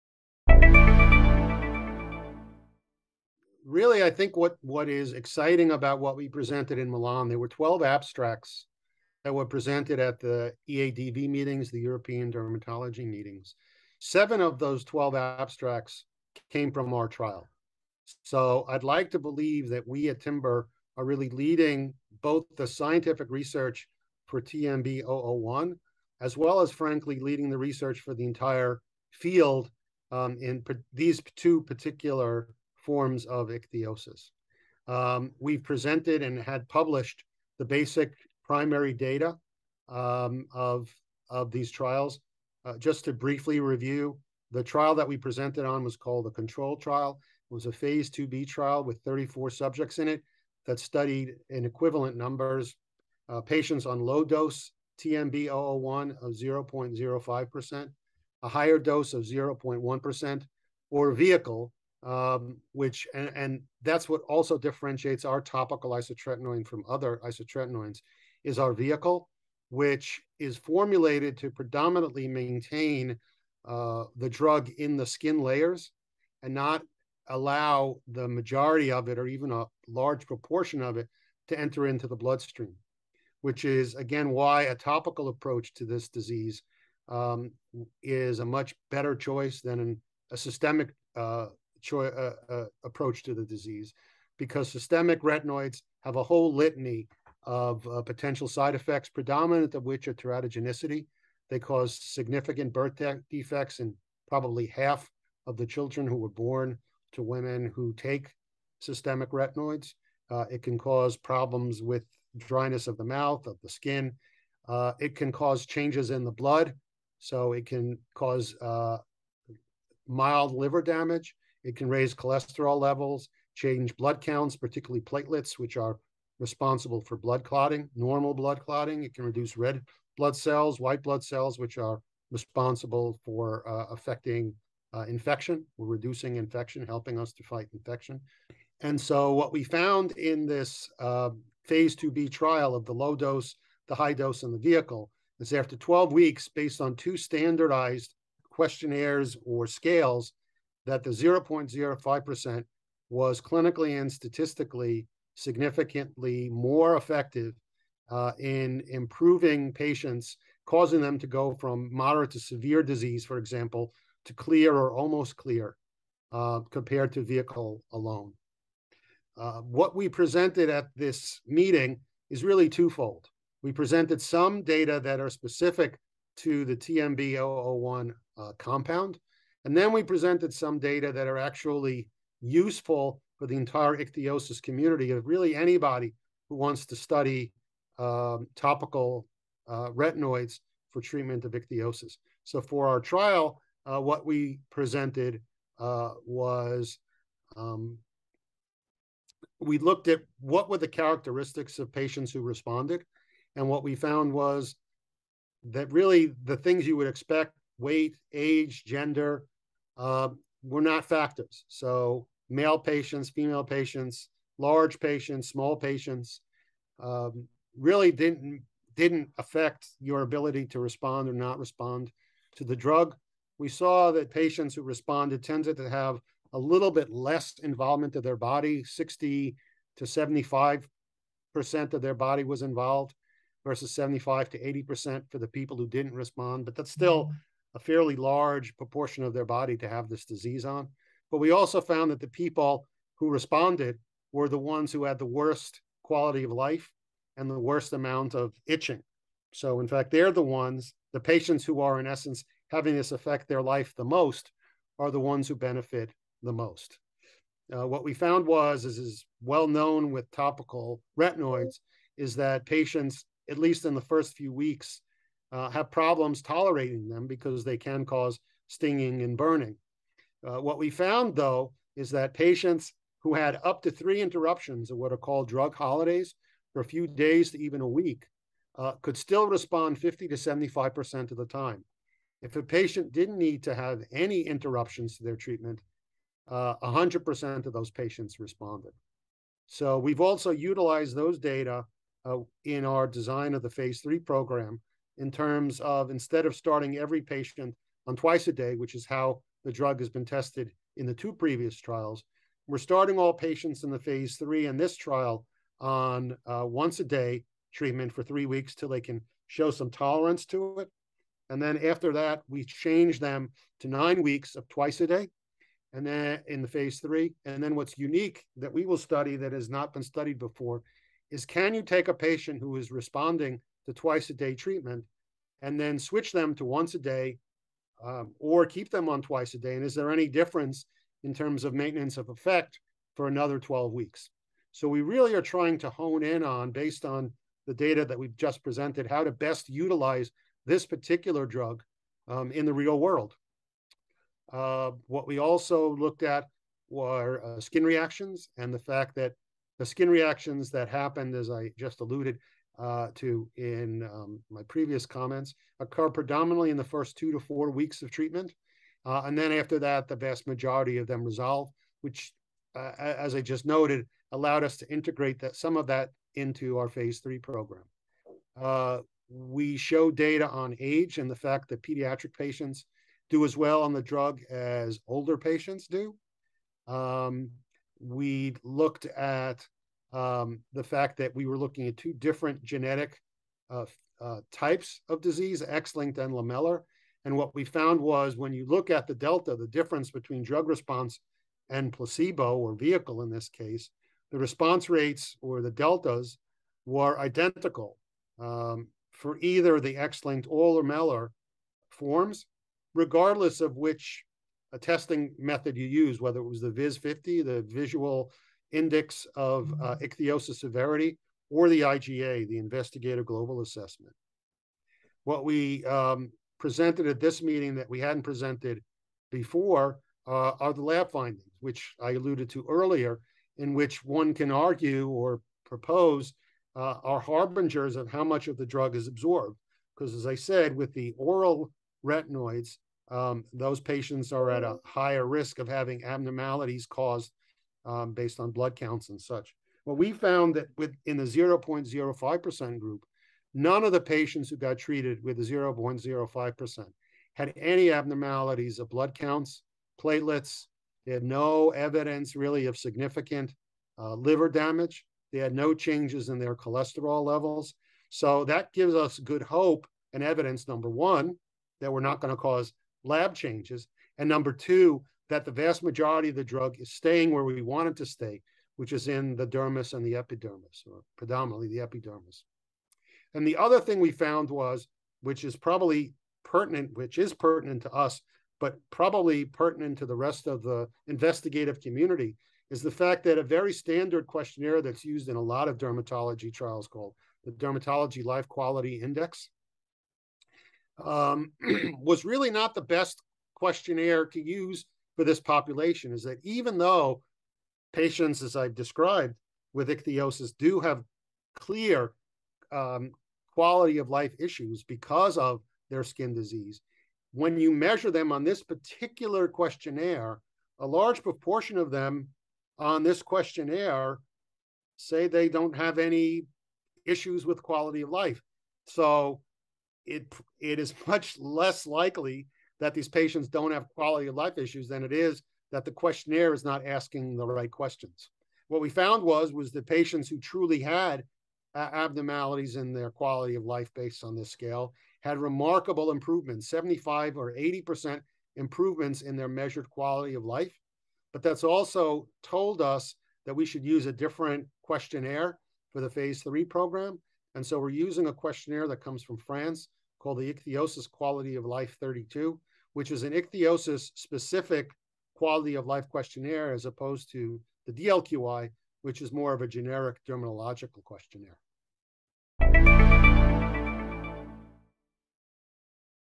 New Phase 2b Data With Topical Ichthyosis To Treat Congenital Ichthyosis Presented
Alan M. Mendelsohn, MD, Executive Vice President and Chief Medical Officer at Timber Pharmaceuticals, discusses the positive clinical data from the completed phase 2b CONTROL study of TMB-001, an investigational treatment for congenital ichthyosis. These data were recently presented at the 31st European Academy of Dermatology and Venereology (EADV) Congress 2022.

Congenital ichthyosis is a rare genetic skin disorder characterized by dry, thickened, and scaling skin. Individuals with this condition may experience limited range in motion, chronic itching, an inability to sweat, and increased risk of infections.

TMB-001 is a topical isotretinoin that has been formulated using the Timber’s patented IPEG™ delivery system, for the treatment of moderate to severe forms of congenital ichthyosis.

As Dr. Mendelsohn explains, Timber Pharmaceuticals recently presented 7 abstracts detailing positive clinical data from the completed phase 2b CONTROL study at EADV 2022. The CONTROL study was a randomized, double-blind, vehicle-controlled study designed to assess the efficacy and safety of two concentrations of TMB-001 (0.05% and 0.1% isotretinoin) for the treatment of two distinct subtypes of moderate-to-severe congenital ichthyosis (X-linked recessive and lamellar ichthyosis) in patients 9 years old or older. Subjects applied TMB-001 twice daily for 12 weeks. The primary endpoint was the reduction of targeted ichthyosis severity, determined by a ≥50% reduction in the validated Visual Index for Ichthyosis Severity (VIIS) scaling score. Secondary endpoints included reduction in overall ichthyosis severity, as measured by a two-point improvement using the Investigator Global Assessment (IGA) scale.
Topline results from the CONTROL trial were announced October 2021 and demonstrated that TMB-001 0.05% was significantly more effective in reducing disease severity in patients with moderate-to-severe congenital ichthyosis compared to vehicle alone. Additionally, TMB-001 was found to be well-tolerated.

Highlights from the data presented at EADV 2022 include:
• Age, gender, and body mass index did not significantly influence response rates to TMB-001.
• Participants who had up to 3 drug interruptions lasting up to a week due to difficulty tolerating TMB-001 were still able to respond to the drug 50-75% of the time.
• All (100%) patients who did not require a drug interruption responded to treatment with TMB-001.
• Skin reactions to TMB-001 typically occurred within the first 2-4 weeks of treatment.
• Disease subtype (X-linked or lamellar ichthyosis) did not impact efficacy of TMB-001
• In the cohort treated with TMB-001 0.05%, none of the patients had any abnormalities in their blood counts or platelets, nor significant liver damage, nor changes in cholesterol levels.
• The commonly used quality of life questionnaire, Dermatology Life Quality Index, may not be the best tool as it may underestimate the burden of disease on these patients’ lives.

The data presented at EADV 2022 have informed the study design of the phase 3 ASCEND clinical trial, which is evaluating TMB-001 in the same subtypes of congenital ichthyosis.